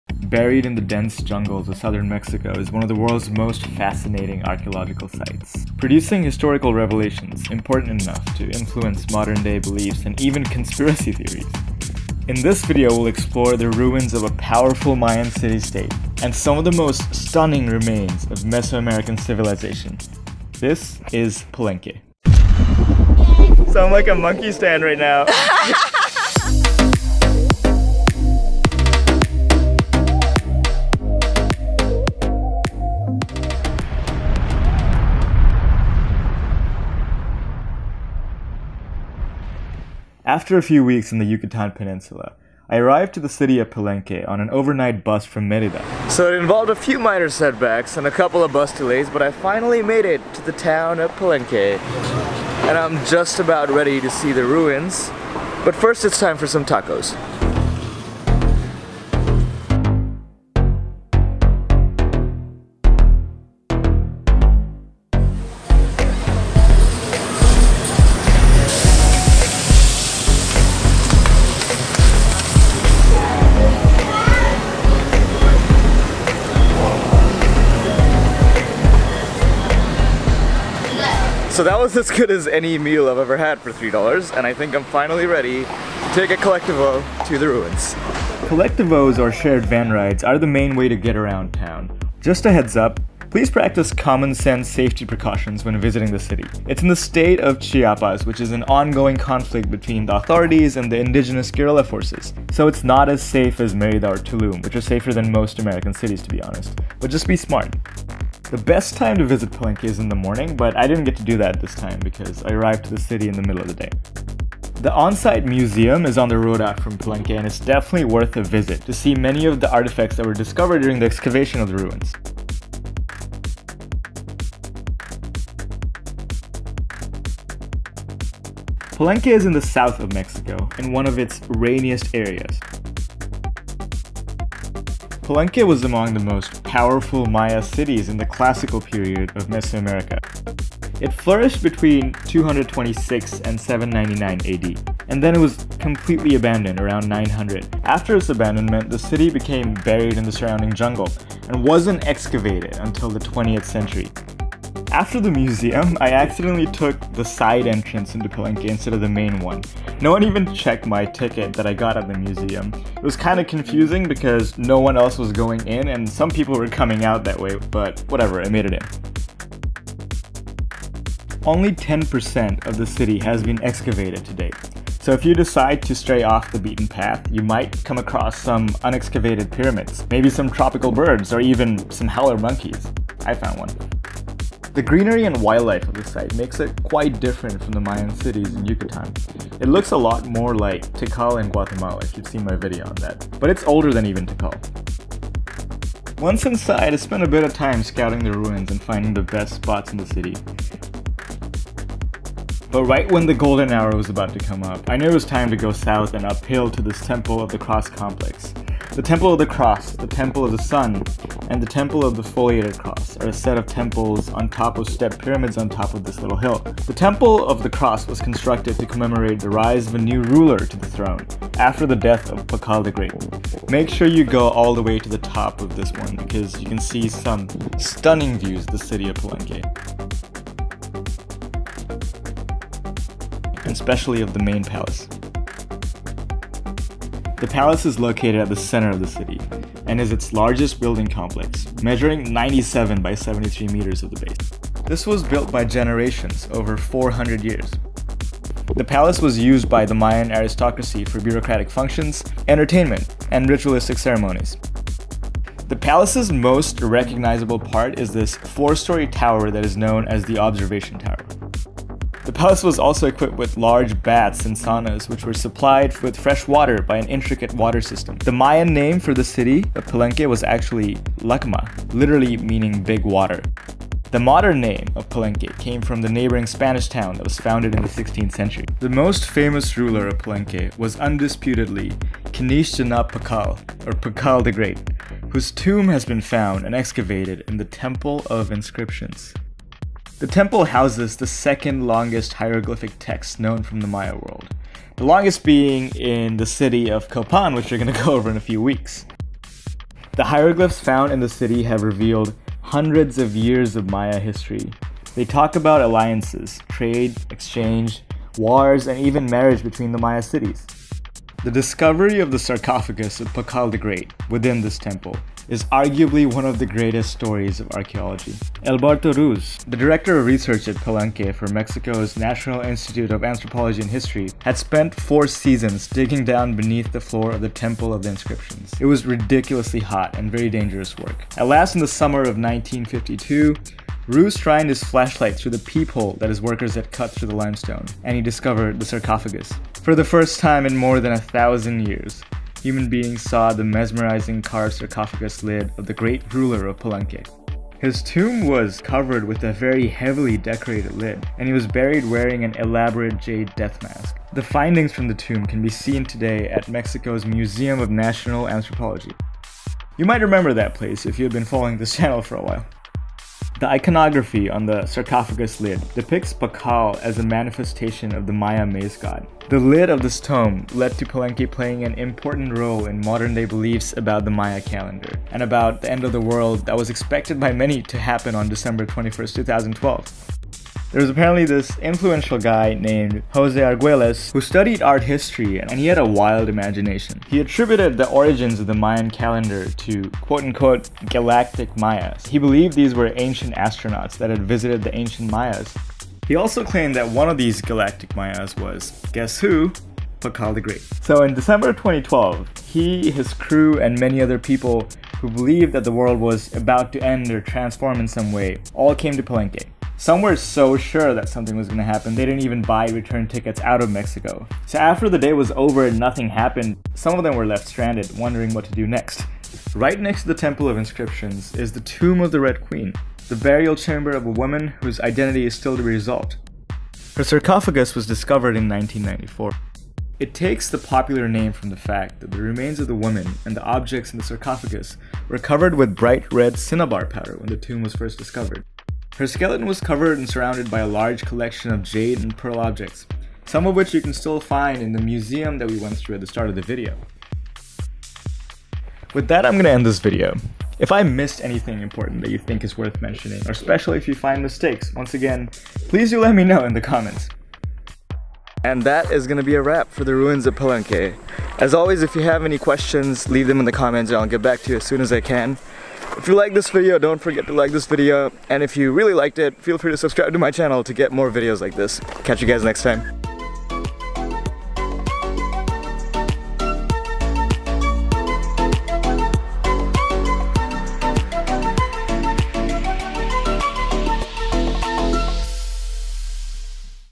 Doomsday Conspiracies Began Here 🇲🇽
I headed to the ruins of Palenque, Mexico's prettiest Mayan Ruins during my backpacking journey, and made a detailed travel guide to former city-state in Mexico's southern state of Chiapas. This should be helpful to you if you are planning a trip to the site or just wanting to know more about the impressive city, its architecture, history and its legends

Gear:
(sorry about low resolution, accidentally recorded most of this trip in 720p instead of 1080)
Lens: Sigma 17-50mm f 2.5

As always, leave any questions in the comment. Any constructive feedback on the video or any necessary corrections/additional information is also welcome in the comments. Hope you enjoy  Palenque.

0:00 Intro
0:40 To  the ancient city of Palenque
0:55 Time to have some Tacos
1:24 Collectivo to the ruins
At the ancient Mayan site
Top of a pyramid
7:38 Outro